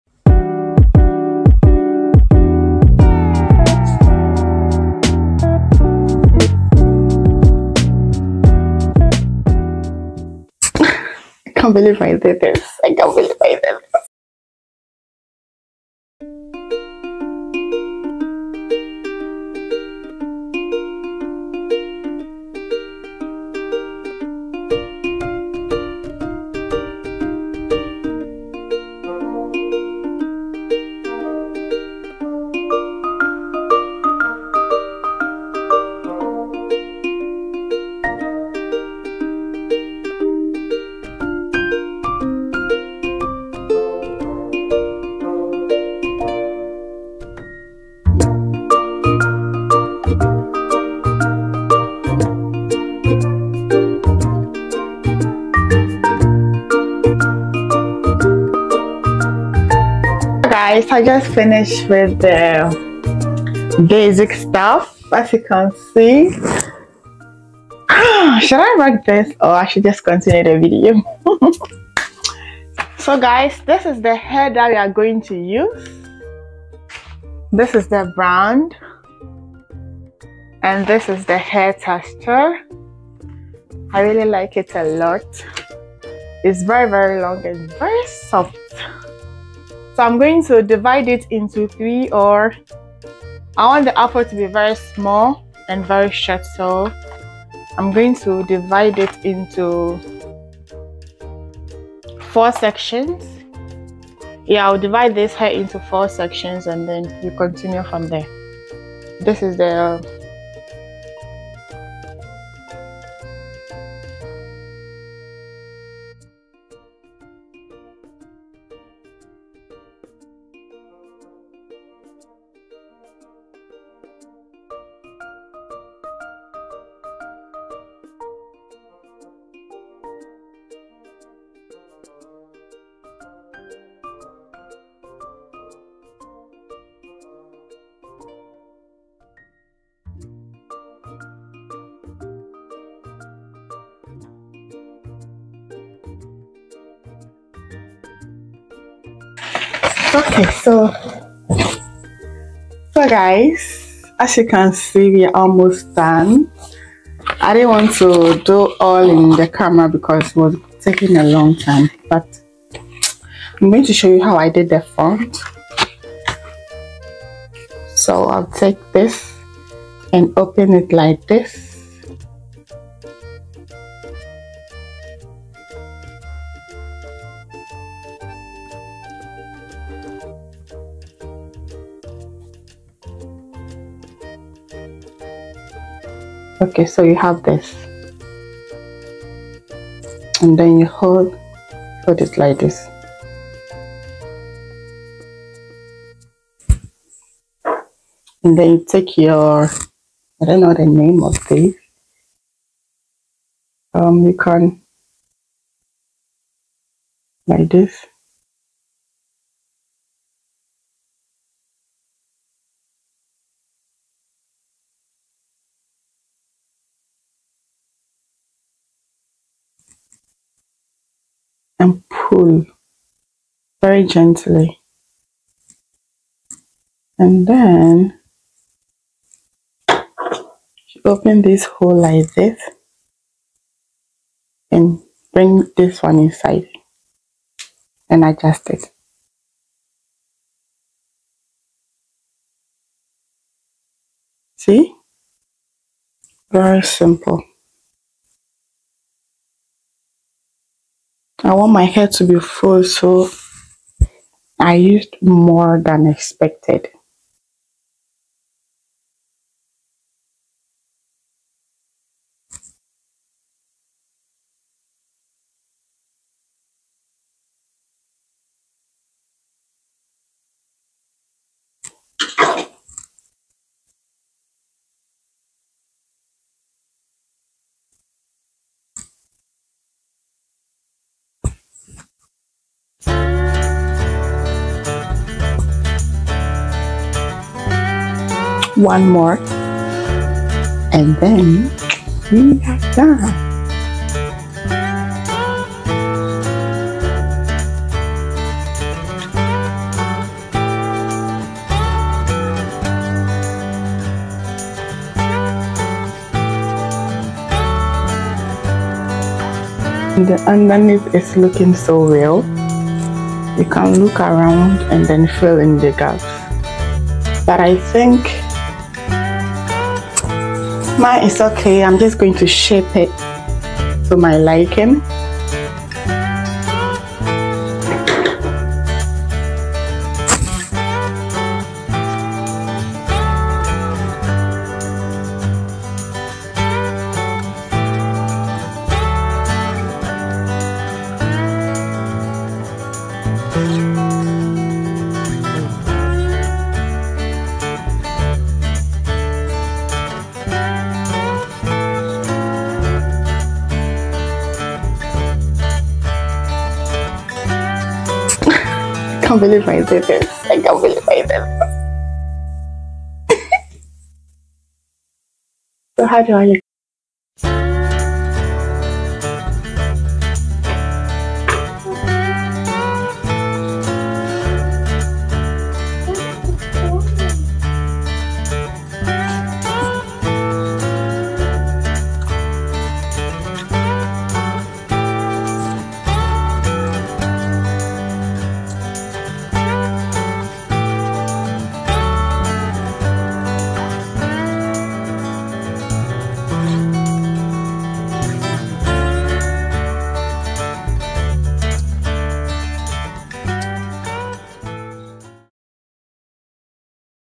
DIY CROCHET BRAIDS | NO CORNROWS | VERY NATURAL Protective Style Hair Tutorial
EASIEST CROCHET BRAIDS | NO CORNROWS | NO BRAIDS | VERY NATURAL Protective Style Hair Tutorial

Hey loves,

Today's video is a detailed Easiest Crochet Braids Tutorial with No cornrows , No Braids literally a whole Braidless Crochet Install in 1 hour on my Short 4C Natural Hair. It is also a very natural look that looks very realistic, perfect quick and easy protective style for those of us that can't cornrow or can't even braid at all. I hope you all enjoy this detailed no cornrows crochet braids tutorial on my short 4C Natural Hair protective style where I show you how to achieve this very Natural crochet look